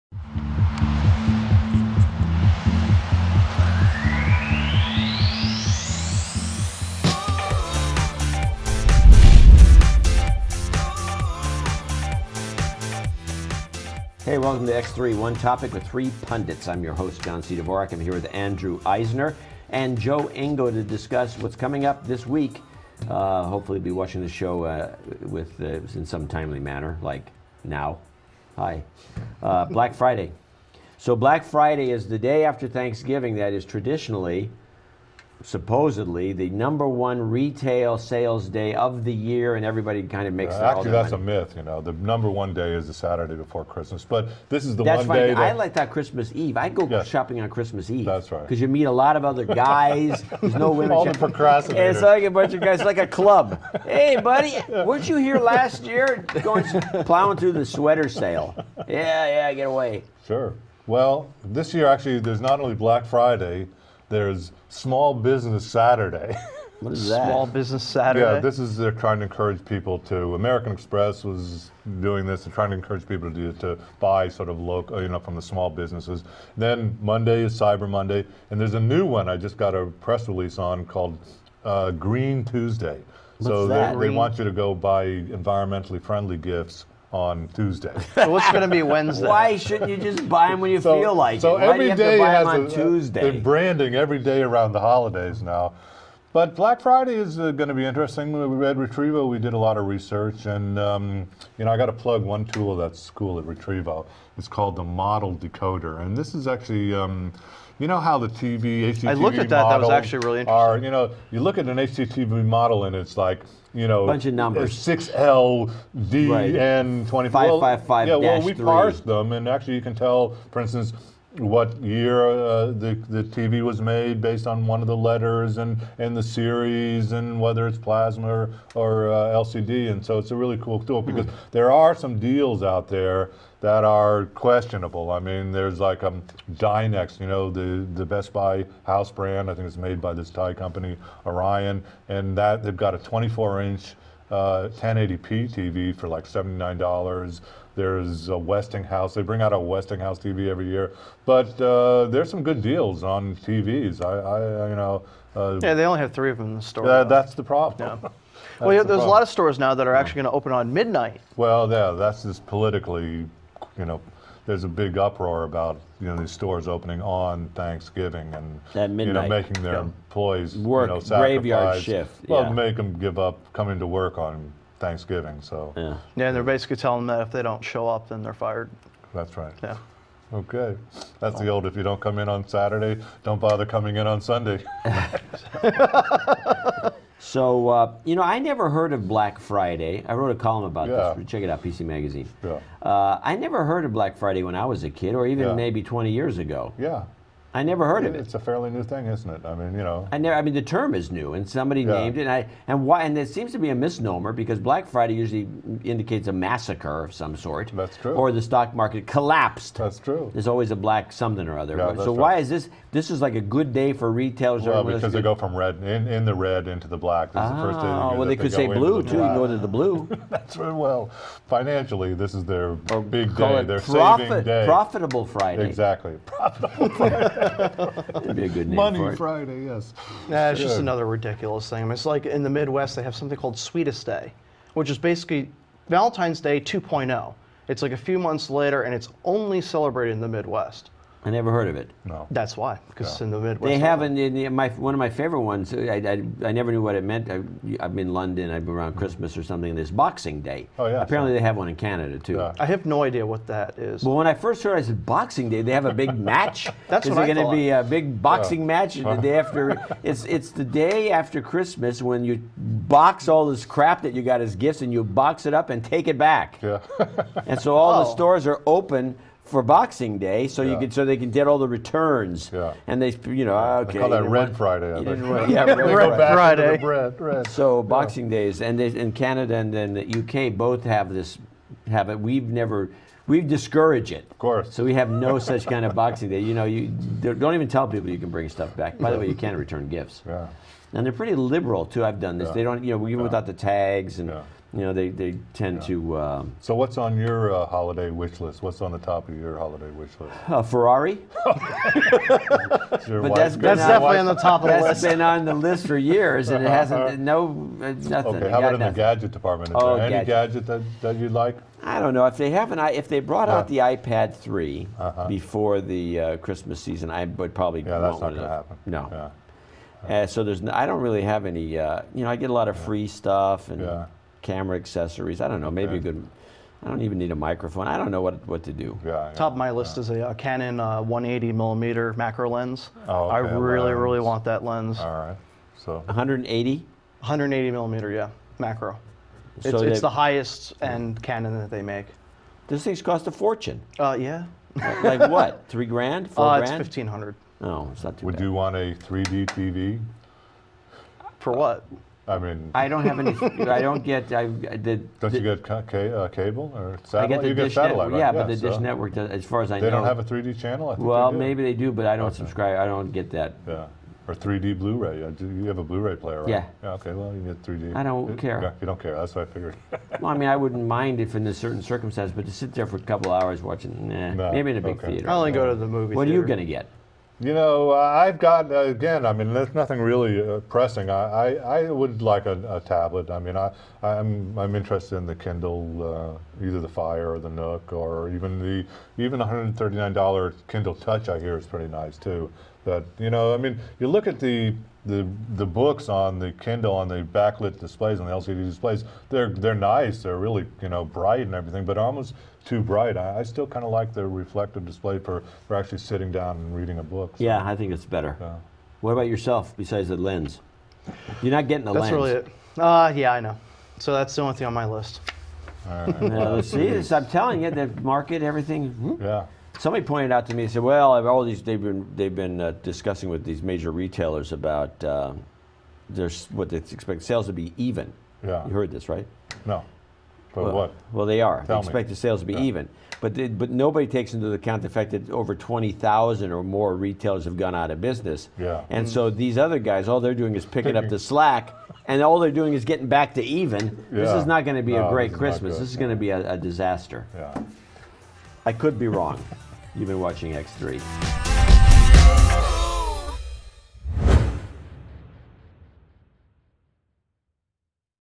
Black Friday/Cyber Monday/Etc...
X3 with John C. Dvorak

Guests:
Andrew Eisner, Retrevo.com
Joseph Engo, IT Specialist

Topic:
X3: Black Friday/Cyber Monday/Etc...

John, Andrew, and Joe talk about Black Friday, and the other days the that have sprung up around it.